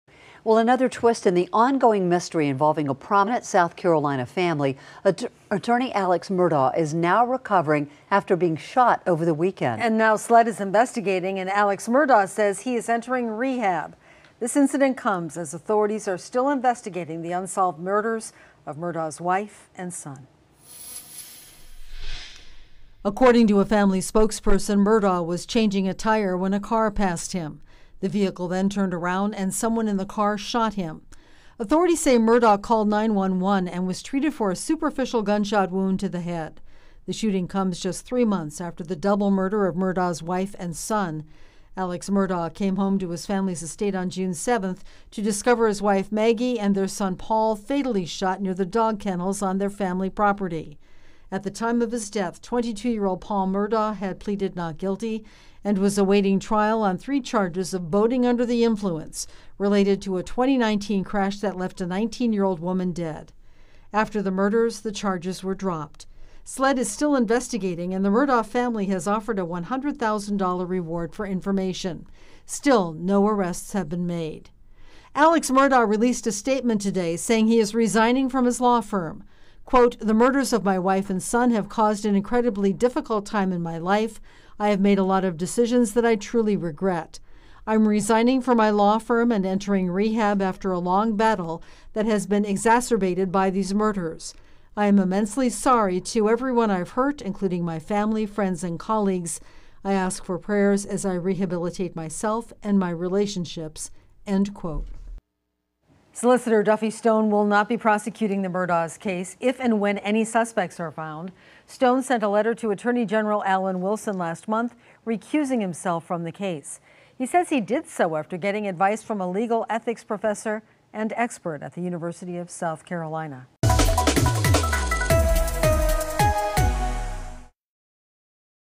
Alex Murdaugh releases statement after being shot in head
Alex Murdaugh, the prominent South Carolina attorney whose wife and son were shot and killed earlier this summer, released a statement Monday after he was reportedly shot in the head.